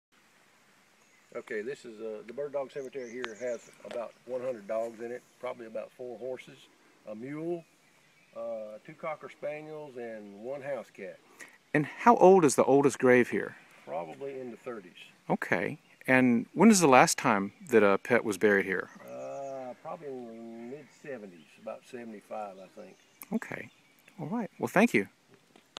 Di-Lane Bird Dog Cemetery
The South's sporting past is memorialized in the bird dog cemetery at Di-Lane Plantation.